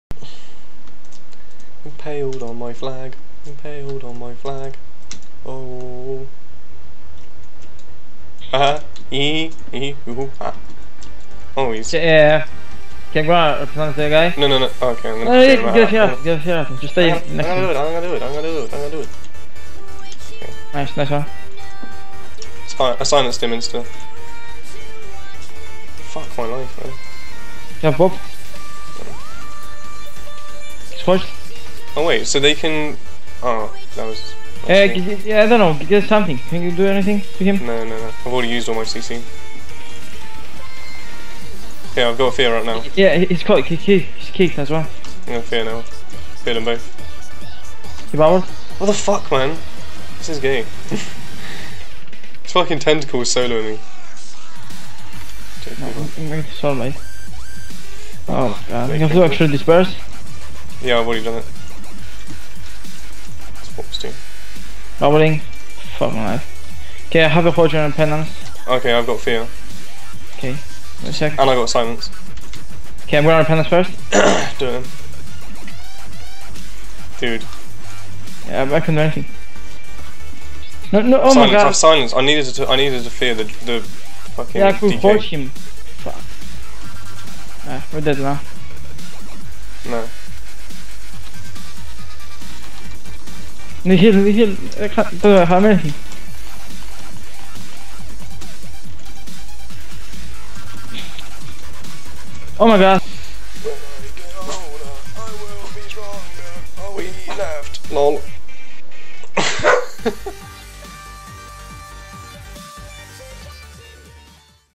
Spriest And Ret Go 2v2 | Spriest PoV | Game 1
These are a few of my first games in arena with my good friend Menikeas..
Hope you enjoy.. there is alot more to come!